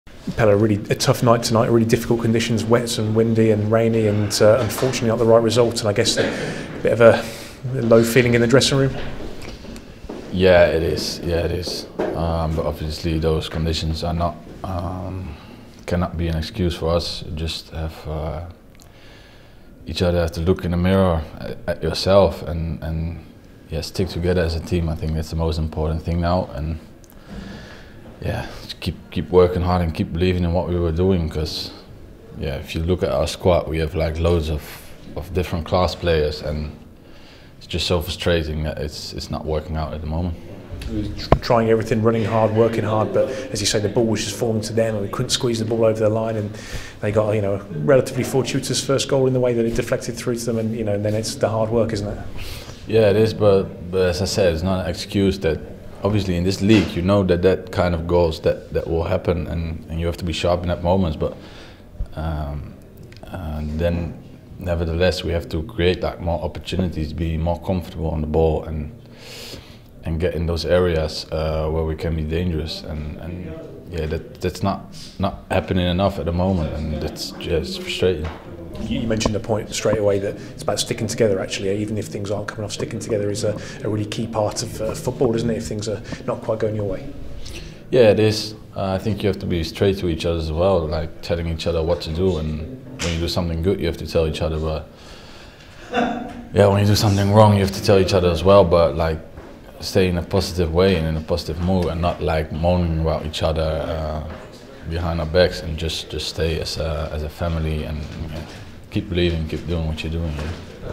Pelle Clement on Blues defeat
Royals attacker Pelle Clement reflects on a disappointing night for the boys after defeat against Birmingham City.